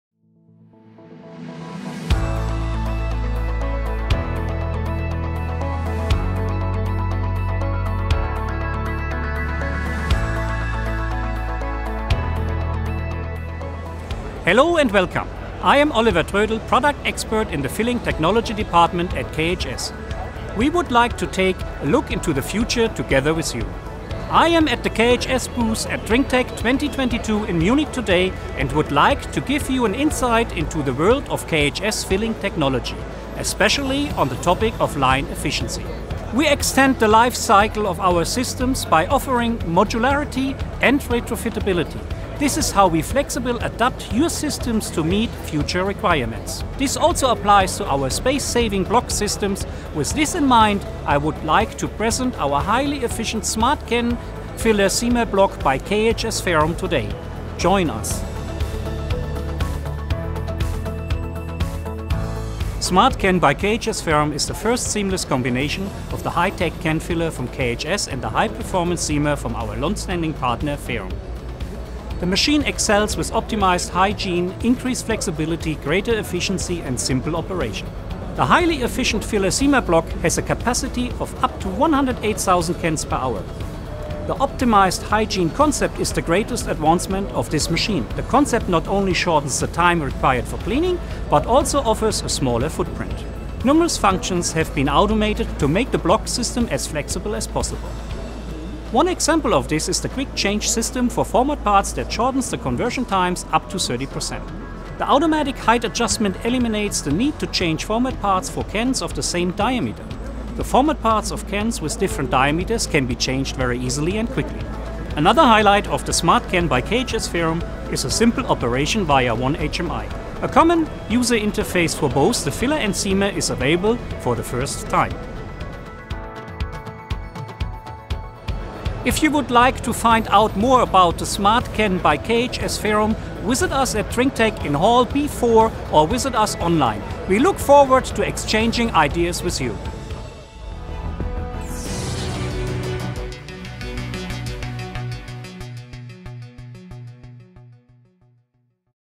Highly efficient filler/seamer block SmartCan by KHS/Ferrum
The SmartCan by KHS/Ferrum is a highly efficient filler/seamer block and is the first seamless combination of the high-tech can filler from KHS and the high-performance seamer from our longstanding partner Ferrum. The machine impresses with optimized hygiene, greater flexibility, higher efficiency and simple operation and has an output of up to 108,000 cans per hour.

If you would like to find out more about the SmartCan by KHS/Ferrum, visit us

Would you like more news from the world of KHS?